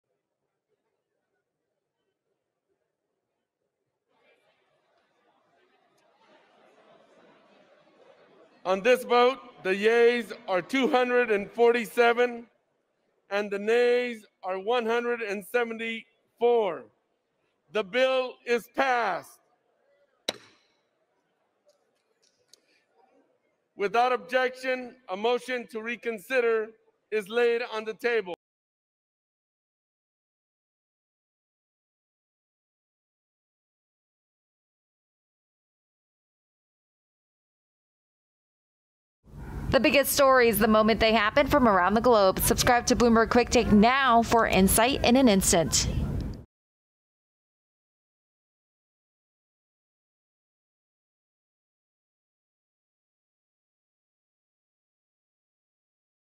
House Passes Bill to Grant Legal Status to Undocumented Farmworkers
The U.S. House on Thursday voted 247-174 to pass a bipartisan bill that would allow an estimated 1 million undocumented farmworkers and their children to gain legal status through continued employment.

There was more bipartisan support for the Farm Workforce Modernization Act, H.R. 1603, which would provide temporary status for certain agricultural workers. It would create a pathway for workers to get a green card by paying a $1,000 fine and engaging in additional agricultural work, depending on how long they’ve had jobs on U.S. farms. It would also streamline the process of getting a temporary visa for farm work and require electronic employment verification.

Pennsylvania Representative Glenn Thompson, the ranking Republican on the Agriculture Committee, said he was voting for the bill, but he hopes Senate would improve it to better resolve the shortage of legal workers for the nation’s ranchers, farms and dairies.